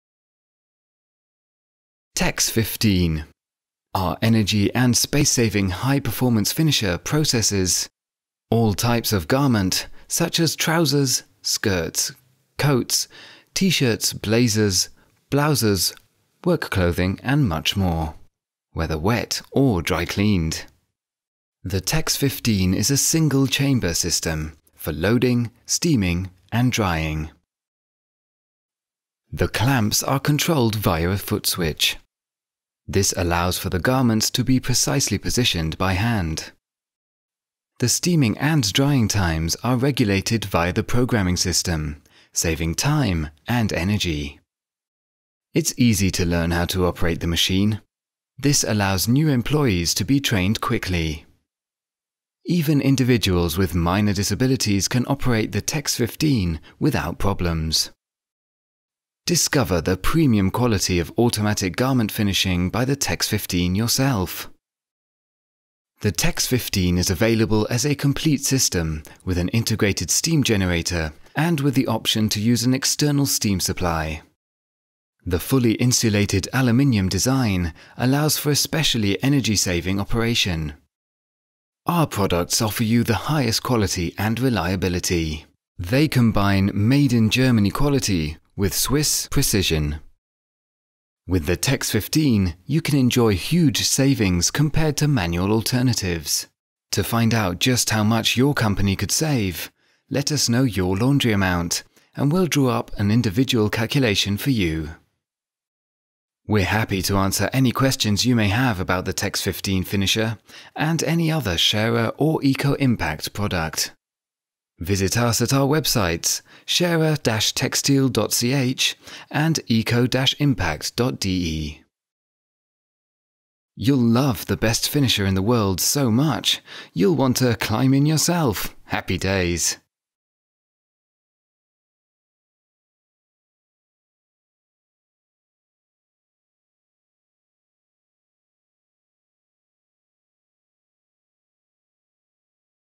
TEX15 High performance finisher
The TEX 15 is a single-chamber system for loading, steaming and drying.

It finishes wet or dry cleaned garments, such as:
trousers, skirts, coats, T-shirts, blazers and sports jackets, blouses, work clothing and much more.

- The clamps are controlled via a foot switch. This allows for the garments to be precisely positioned by hand.
- Steaming and drying times regulated via programming system.
- Operation easy to learn.
- Excellent finishing quality.
- With integrated steam generator or connection to external steam equipment.
- Fully insulated aluminium design.
- Quality products with maximum reliability.

MADE IN GERMANY combined with Swiss precision.

Huge cost savings compared to manual alternatives.
Want to know just how much you could save with the TEX 15?
We’ll be happy to draw up an individual calculation for your company.